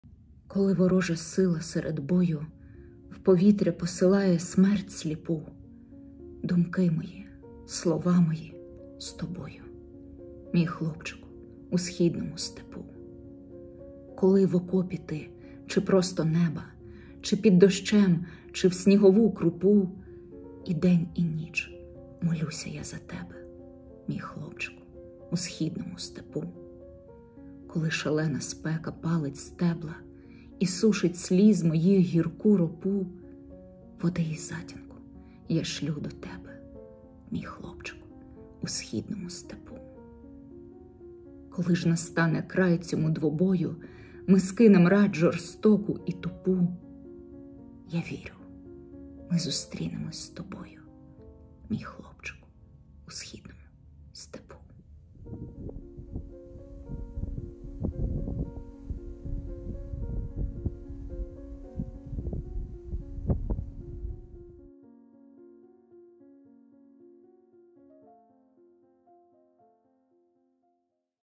Тетяна Шевченко «Мій хлопчику у східному степу» | Марія Гончар
Коли ворожа сила серед бою
в повітря посилає смерть сліпу -
думки мої, слова мої - з тобою,
мій хлопчику у східному степу.

Коли в окопі ти чи просто неба,
чи під дощем, чи в снігову крупу -
і день і ніч молюся я за тебе,

Коли шалена спека палить стебла
і сушить сліз моїх гірку ропу -
води і затінку я шлю до тебе,

Коли ж настане край цьому двобою,
ми скинем рать жорстоку і тупу -
я вірю, ми зустрінемось з тобою,

Тетяна Шевченко
23.07.23

Читає: Марія Гончар